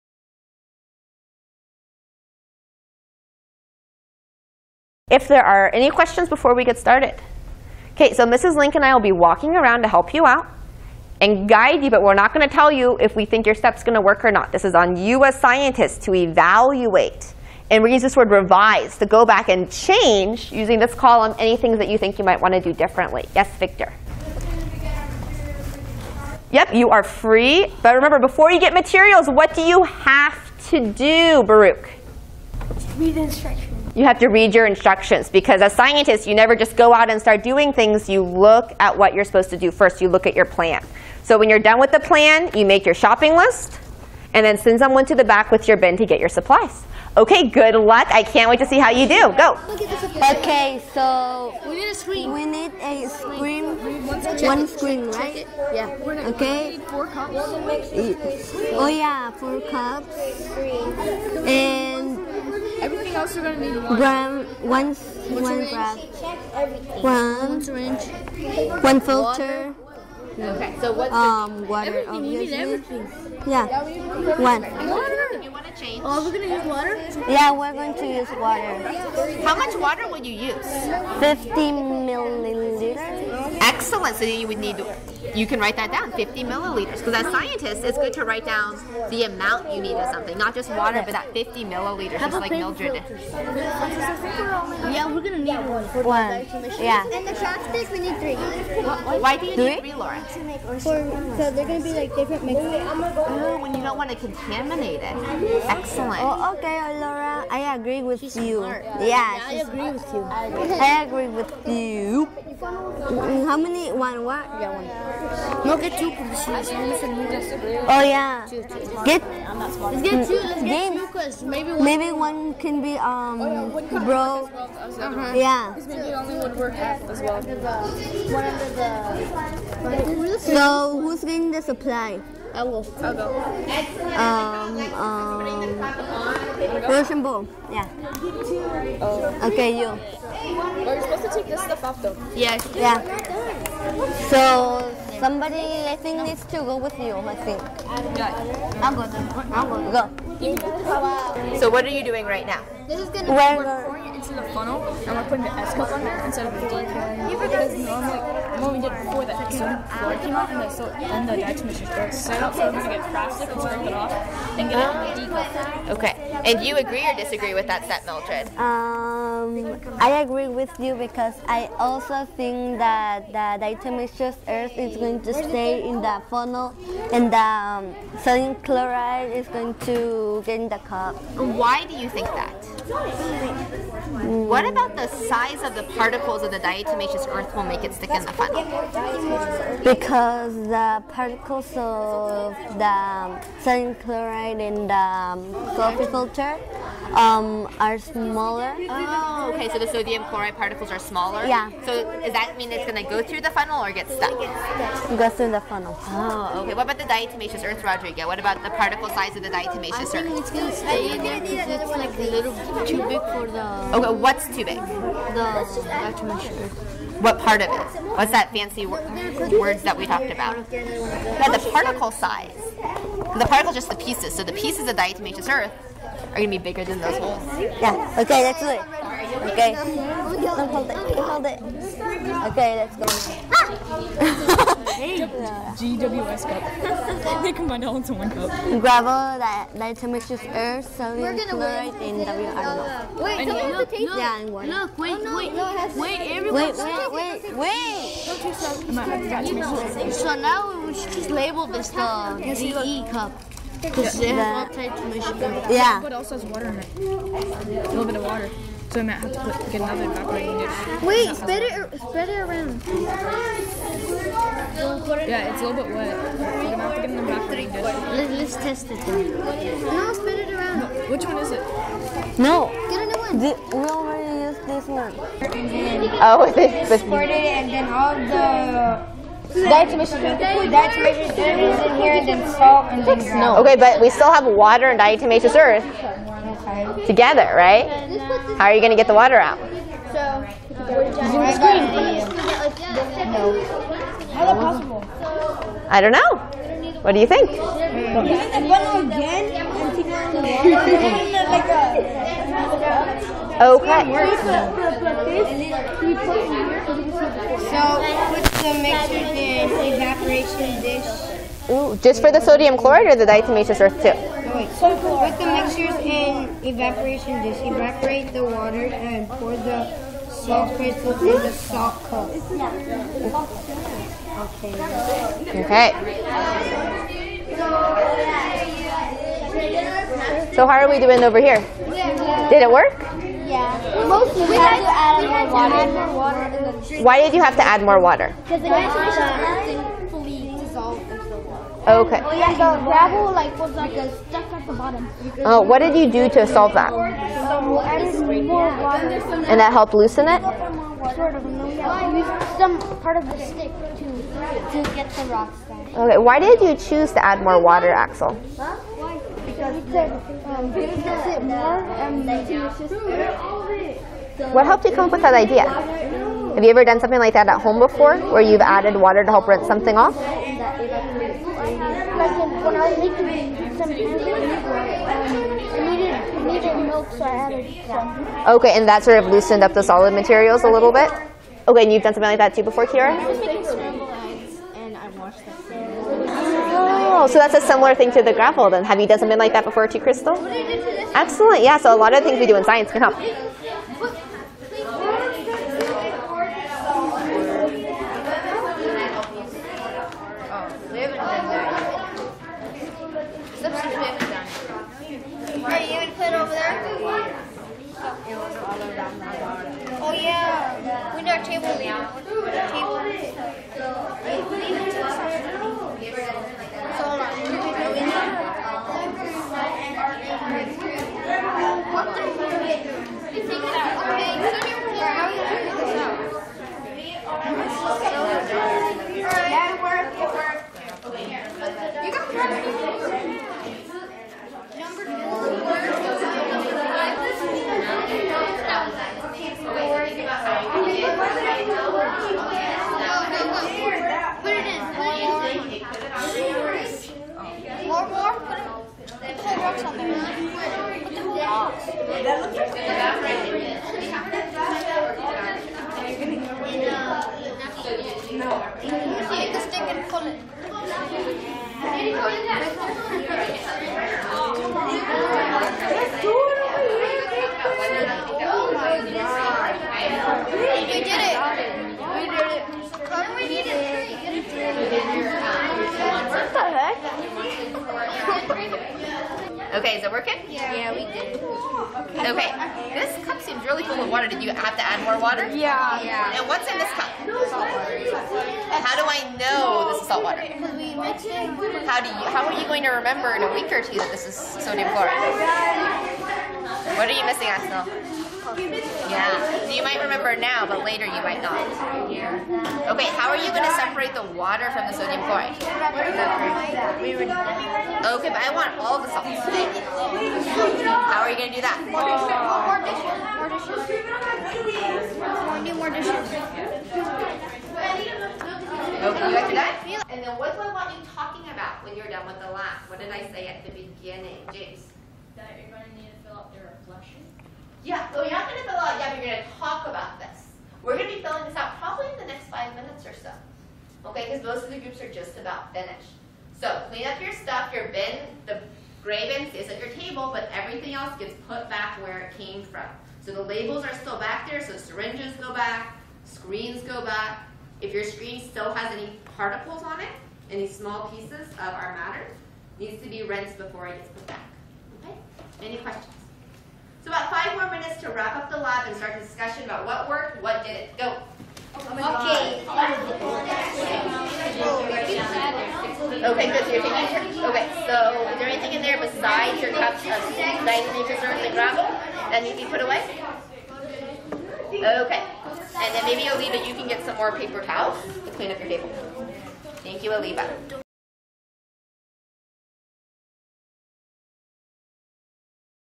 Separating Mixtures Lesson - Part 2 Lab Activity
Gr 5 science lesson - FOSS Mixtures and Solutions Unit
Ms. Adelyn Lyon, Mountain View CA